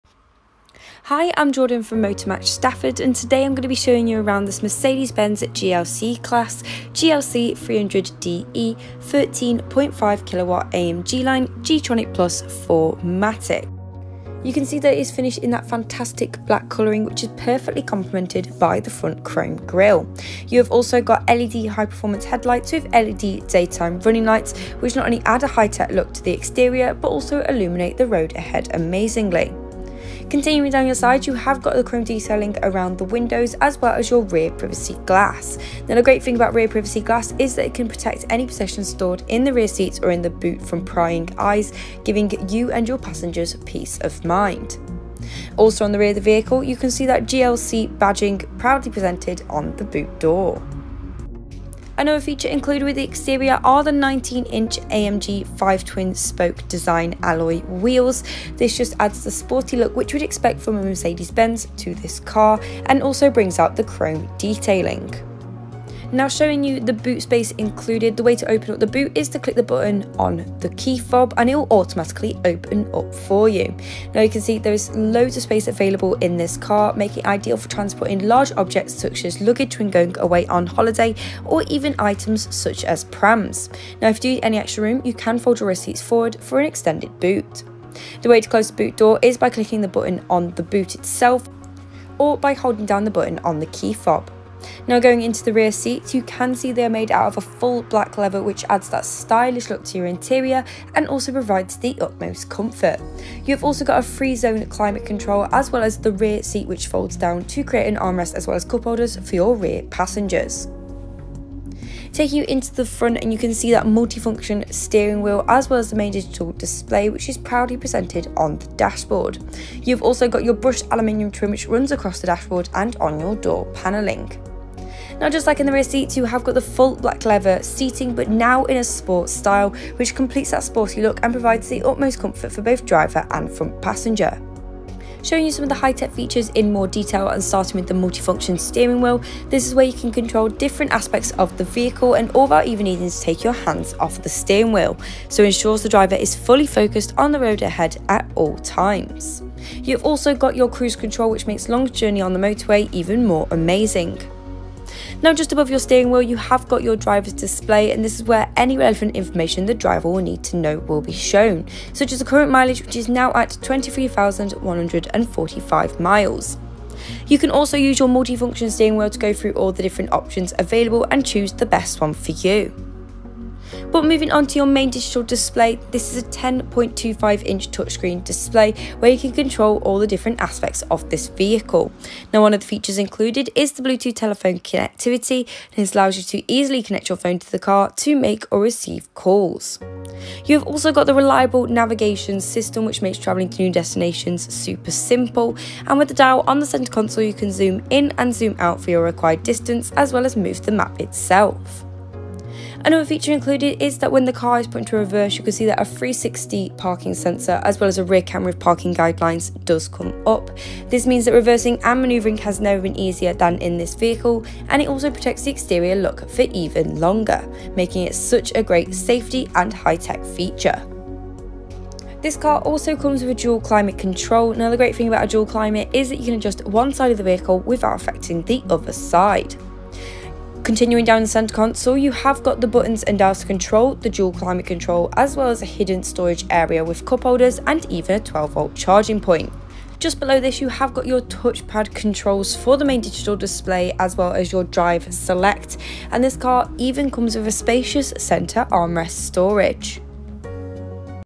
Used Mercedes-Benz GLC Class 2.0 GLC300de 13.5kWh AMG Line G-Tronic+ 4MATIC | Motor Match Stafford
This outstanding Used Mercedes-Benz GLC Class 2.0 GLC300de 13.5kWh AMG Line G-Tronic+ 4MATIC is now available from Motor Match Stafford.

Reg: DY71CTV
Fuel: Diesel Plug-In Hybrid
Gearbox: Automatic

Key features include:
~ Rear Parking Camera with Guidelines
~ 3 Zone Climate

OR

0:00 - Exterior Tour
1:06 - Boot Space Tour
1:35 - Interior Tour
2:15 - High Tech Features